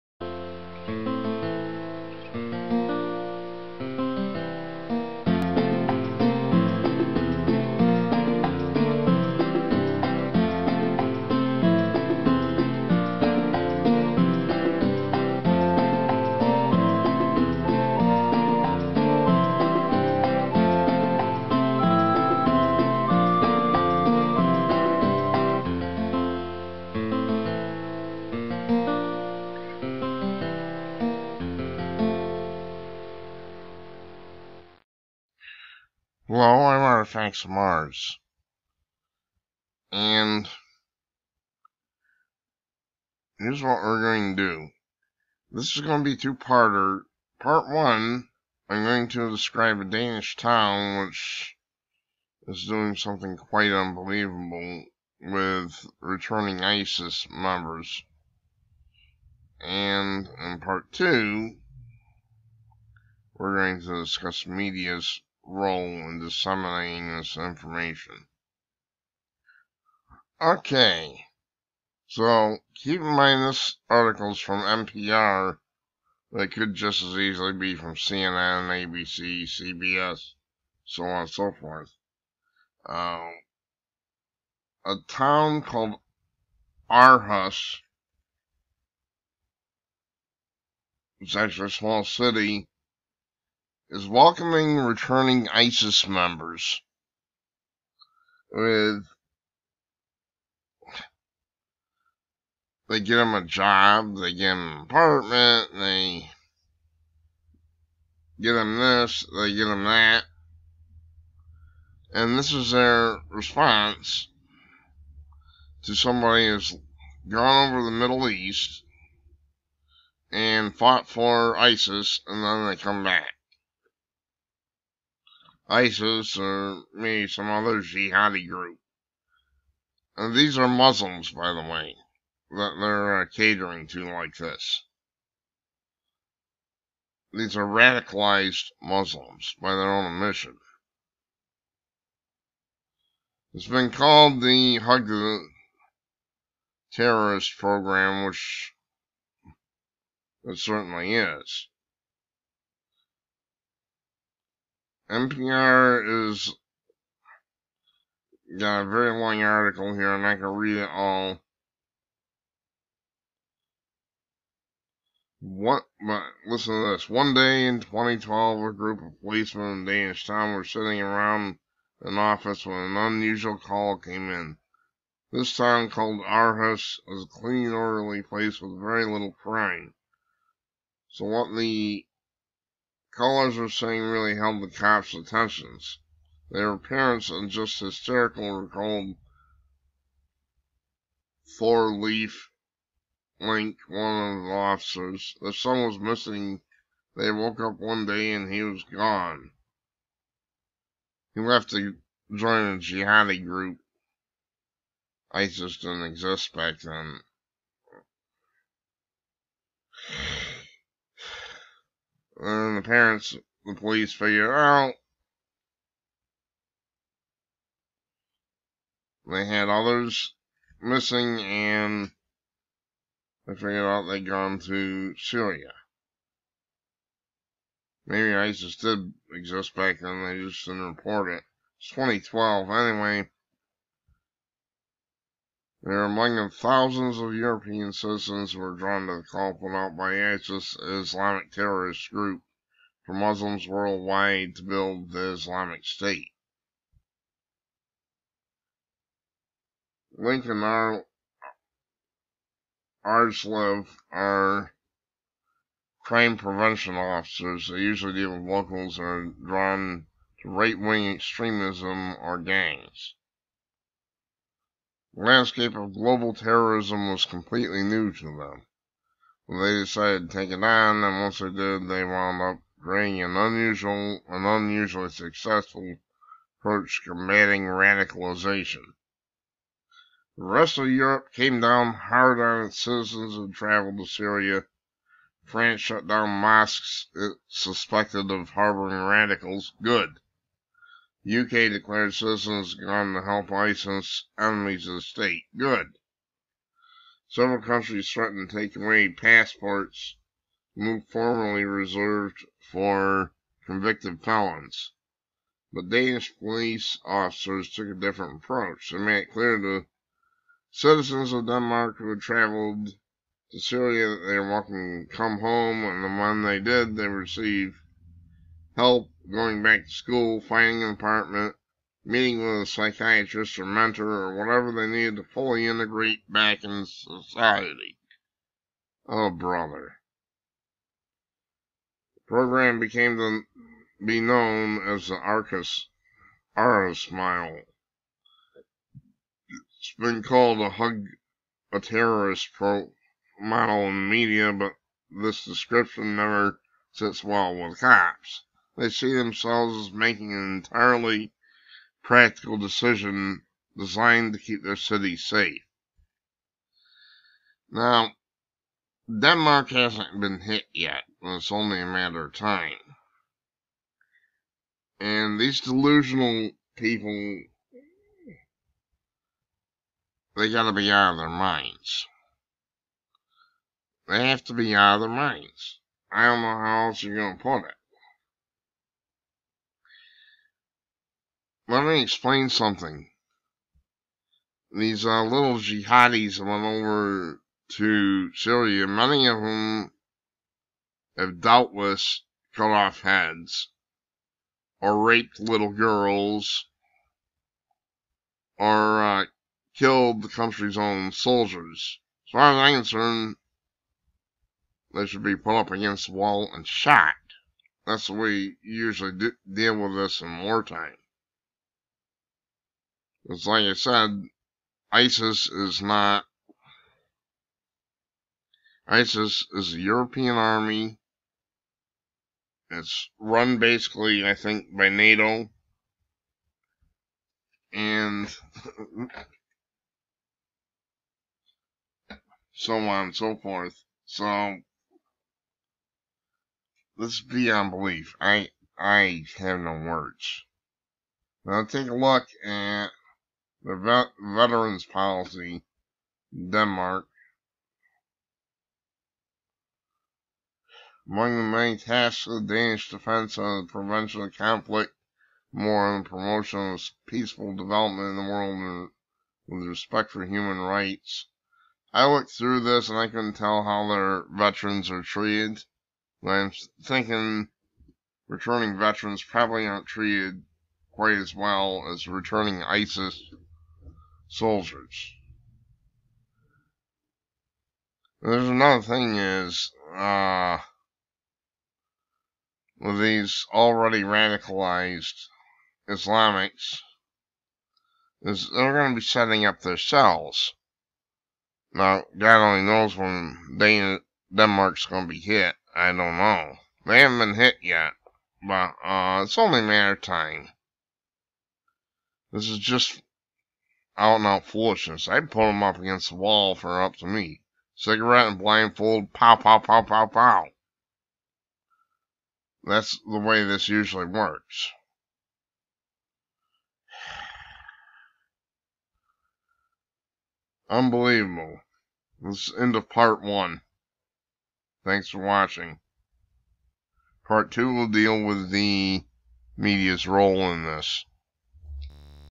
Let's all hug a terrorist, part 1
In Denmark a city welcomes returning Jihadists, helping them get apartments, jobs and psychiatric services/ counseling and so forth.
   Artifactsofmars wants to know how many of these ISIS Jihadists have raped little girls, chopped off heads and so on. This is sheer lunacy!